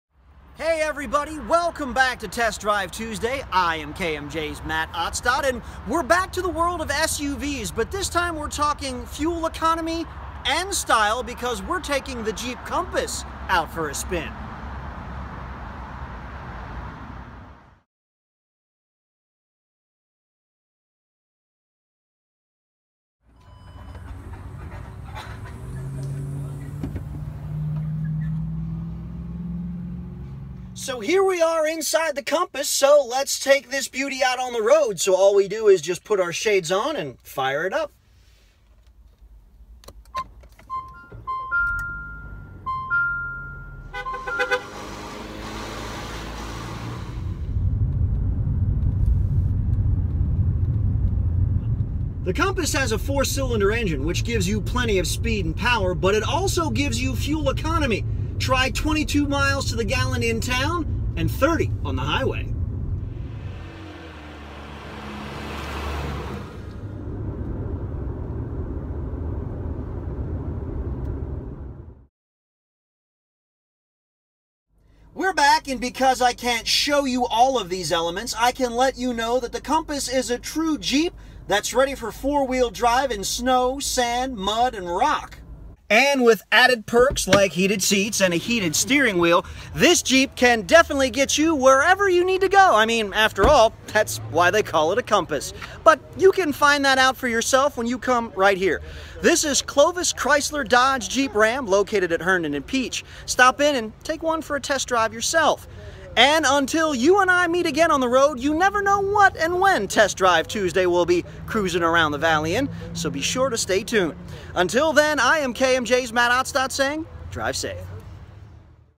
Test Drive Tuesday #12
KMJ's Matt Otstot hits the roads of the Central Valley in an all-new Jeep Compass in the latest Test Drive Tuesday